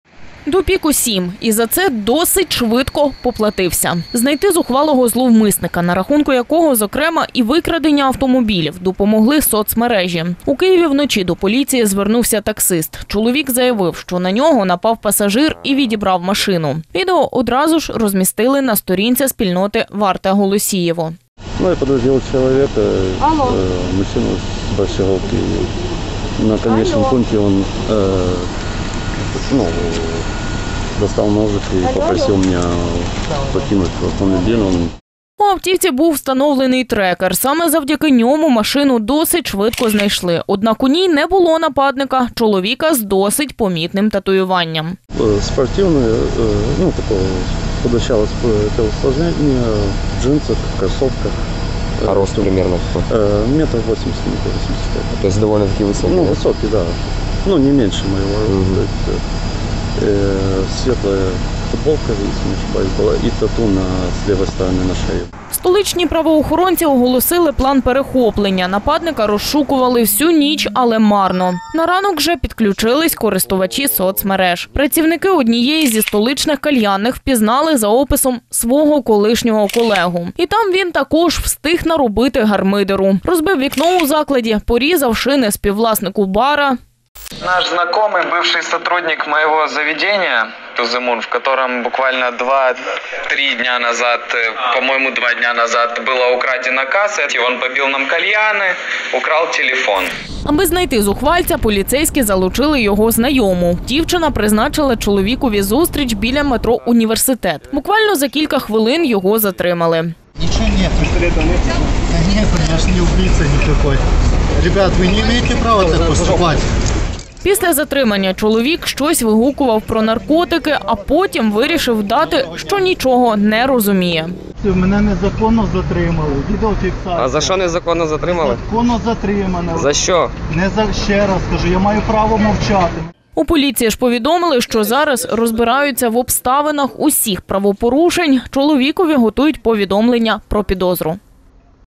У Києві зловмисник швидко поплатився волею через те, що допік своїм знайомим
Допік усім за це й досить швидко поплатився волею. Знайти зухвалого зловмисника, на рахунку якого два злочини, серед яких крадіжка та заволодіння автівкою, допомогли саме соцмережі. У Києві вночі до поліції звернувся таксист. Чоловік заявив, що його авто викрав пасажир. Відео одразу ж розмістили на сторінці спільноти Варта Голосієво.